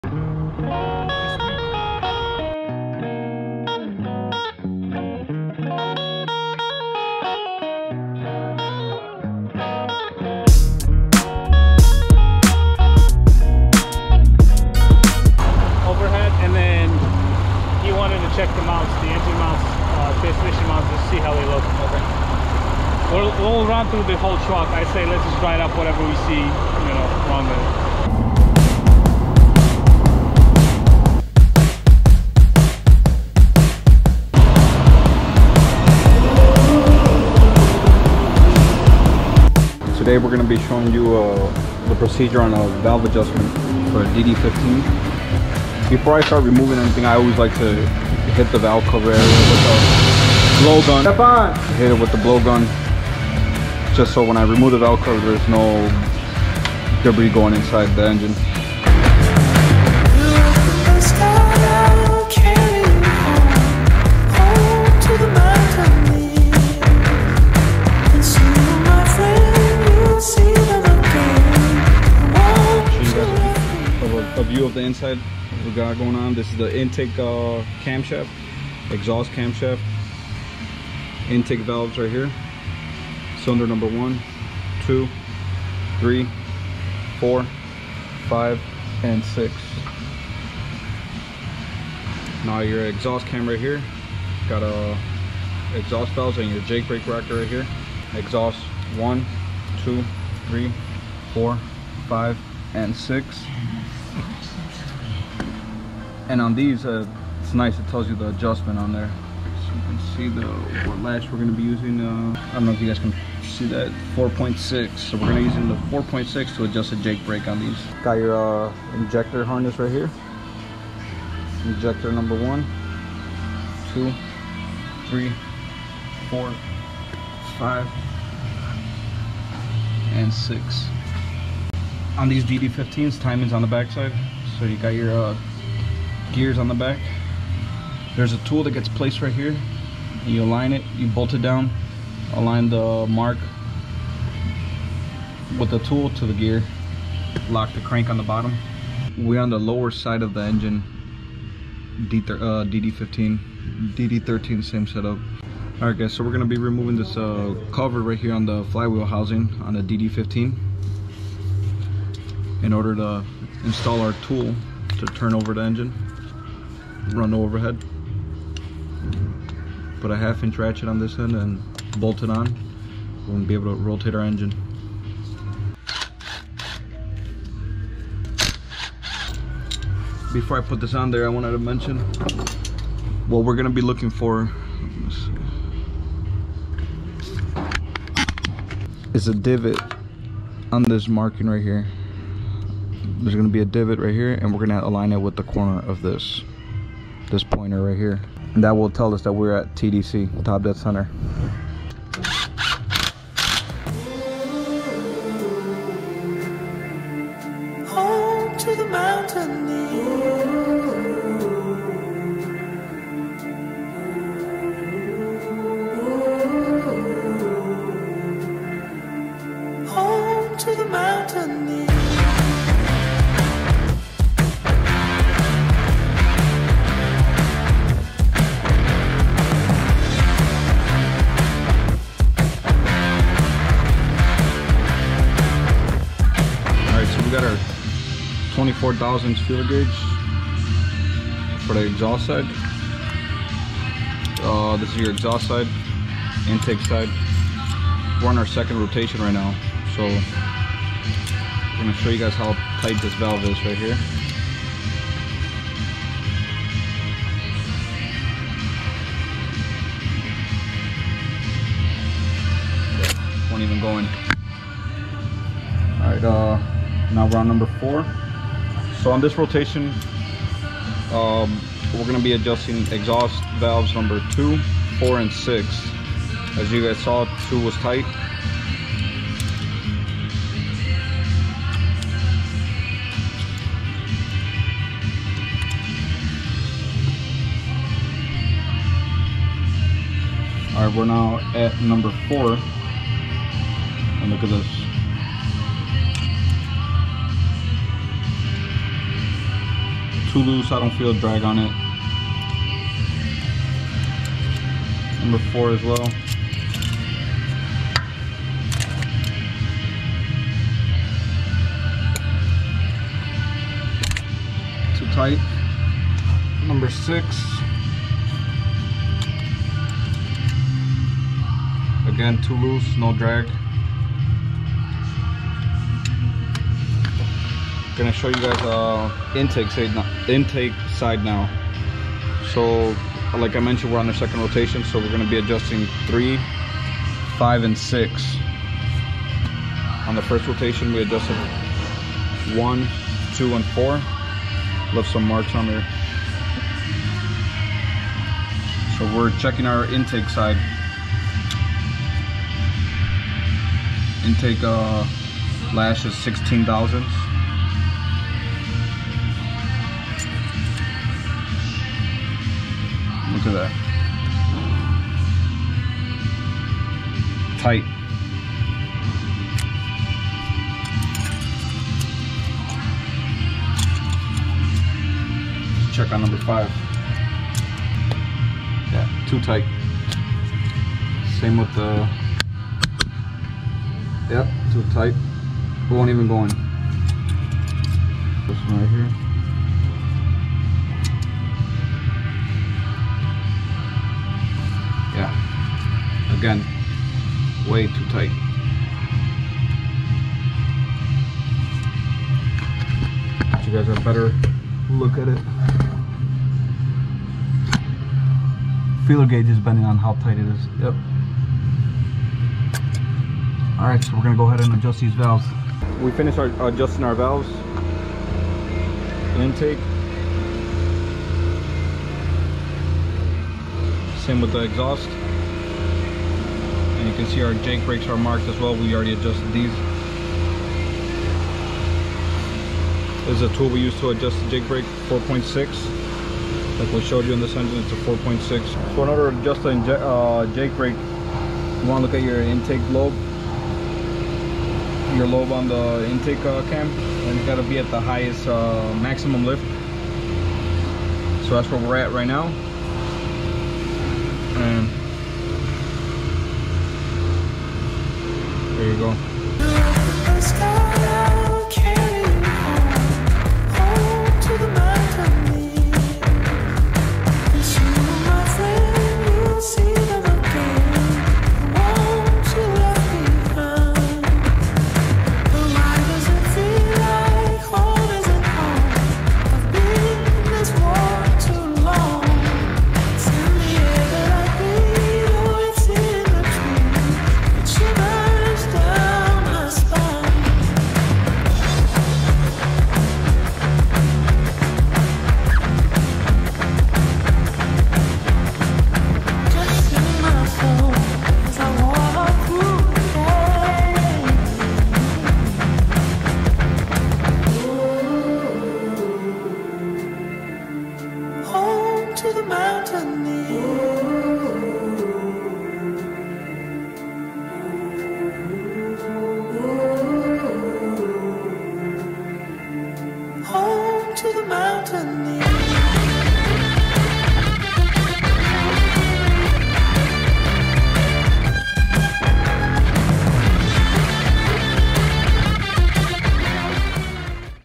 Detroit DD15 overhead valve adjustment. Freightliner Cascadia
Hi Truckers! Valves are a crucial component in the diesel engine when it comes to running at peak performance and ensuring the engine has proper combustion.

In this video, Jose walks you through the process of an overhead (valve adjustment) on a DD15 in a Freightliner Cascadia. Tune in to see how to access the valves, identify the engine components in the overhead, how to test the valves current placement and what we do in an overhead to adjust the valves.